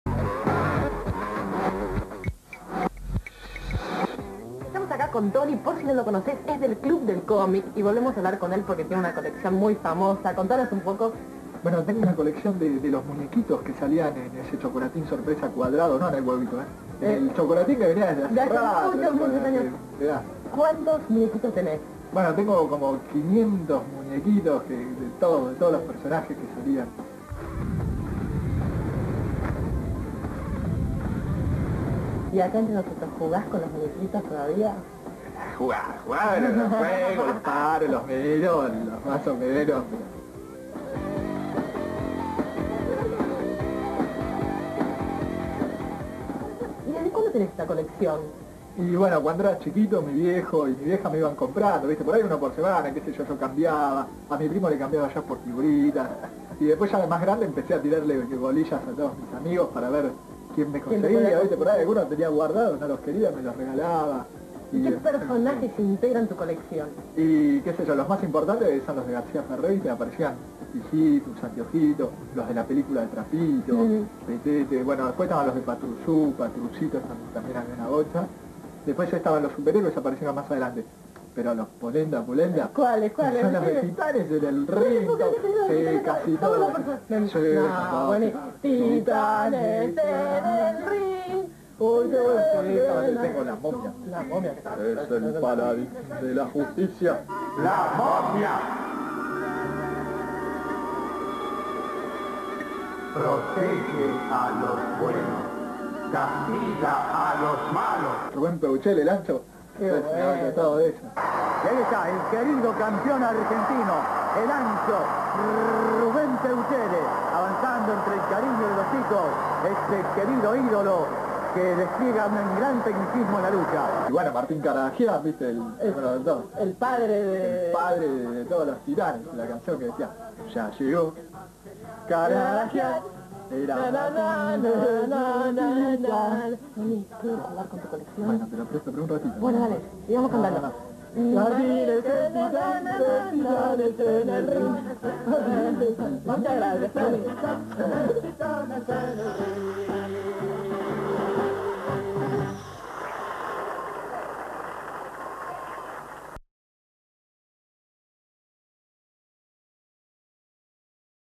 Cablin - Colección Chocolate Jack Garcia Ferrer Titanes en el Ring - DiFilm (1995)
CONSULTAS PARA ADQUIRIR VIDEOS A: - Cablin María Eugenia Molinari Reportaje a Tony del club del cómic sobre colección muñequitos Chocolate Jack Garcia Ferrer Titanes en el Ring.
Fecha: 1995
Duración: 2 minutos 36 segundos.
Código: V-06556